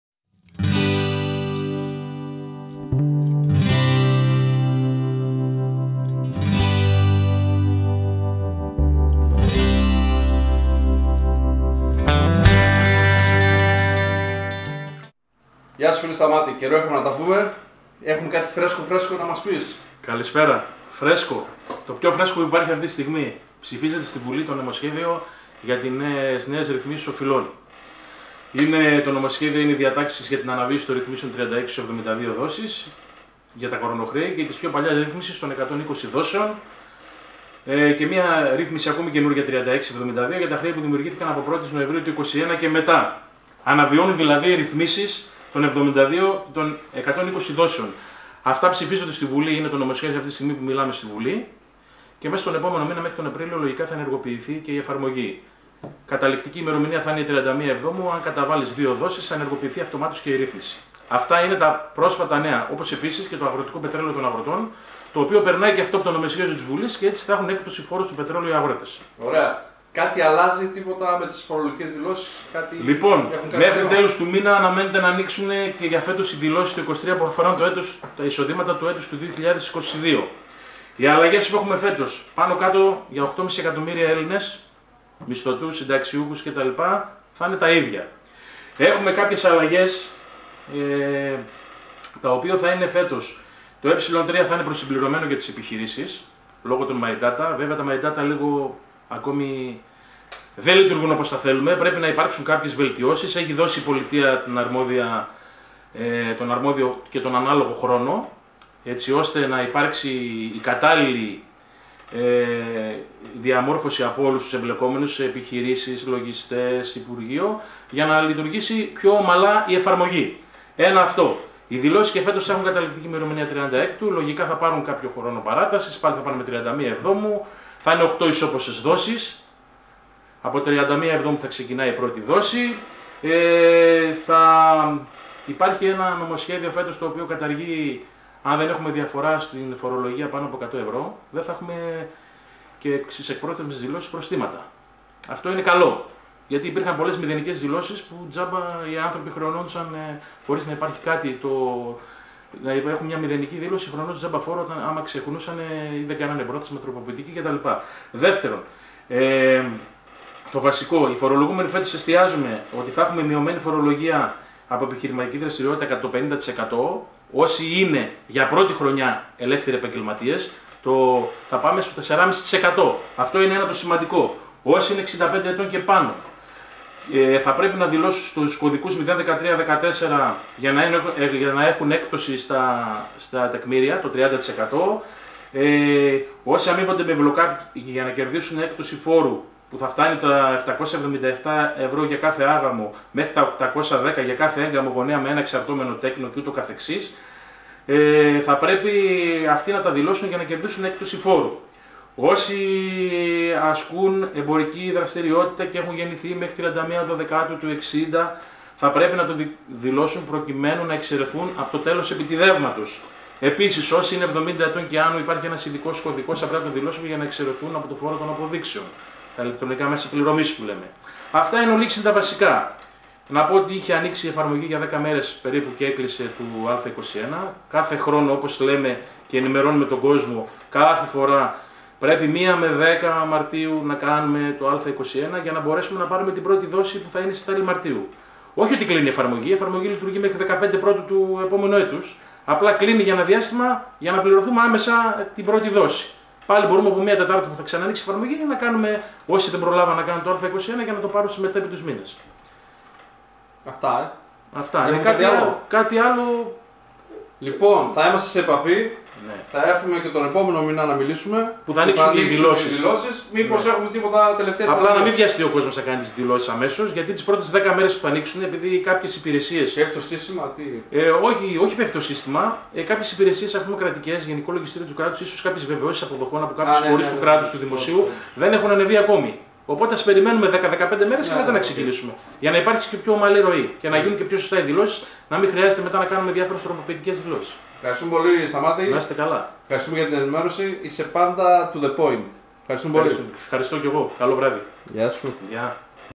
Τι Θα Ψηφιστεί Αυτες Τις Μερες Στην Βουλή Μας Τα Λέει Όλα Ο Σταμάτης Τσιτσιγιάς Λογιστής
Σήμερα πήγαμε από το γραφείο του Σταμάτη και μας ενημέρωσε τι θα ψηφιστεί αυτές  τις μέρες στην Βουλή αλλά και για τις φετινές αλλάγε για την φορολογική δήλωση

Όποιος θέλει να βοηθήσει οικονομικά το κανάλι μας μπορεί να το κάνει στο παρακάτω